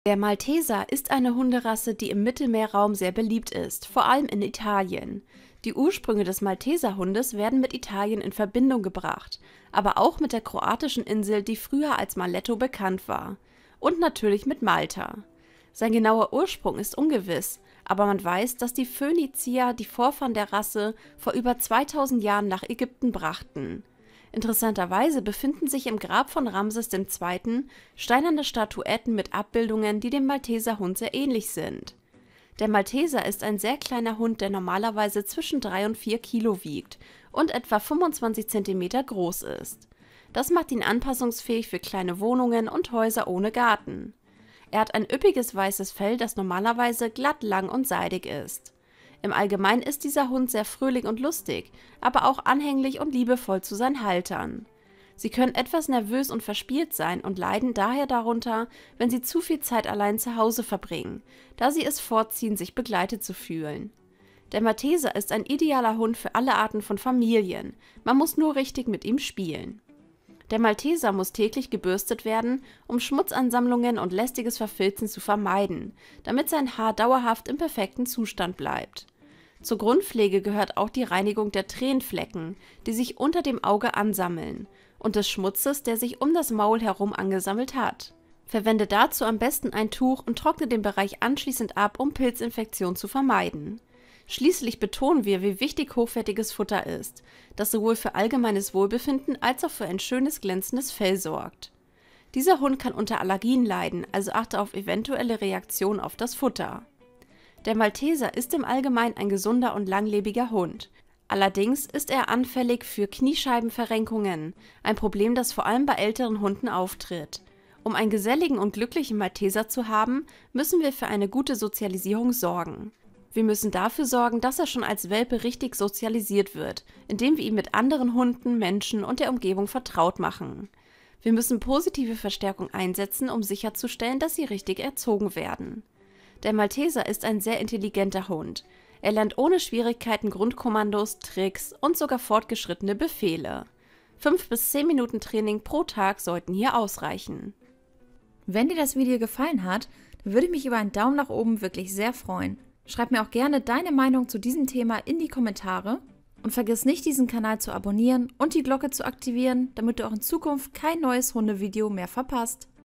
Malteser ▶︎ Charakter, Haltung & Pflege | Rasseportrait
Der Malteser ist eine Hunderasse, die im Mittelmeerraum sehr beliebt ist, vor allem in Italien. Die Ursprünge des Malteserhundes werden mit Italien in Verbindung gebracht, aber auch mit der kroatischen Insel, die früher als Maletto bekannt war. Und natürlich mit Malta. Sein genauer Ursprung ist ungewiss, aber man weiß, dass die Phönizier die Vorfahren der Rasse vor über 2000 Jahren nach Ägypten brachten. Interessanterweise befinden sich im Grab von Ramses dem Zweiten steinerne Statuetten mit Abbildungen, die dem Malteserhund sehr ähnlich sind. In diesem Video verraten wir dir alles Wichtige über diese Rasse!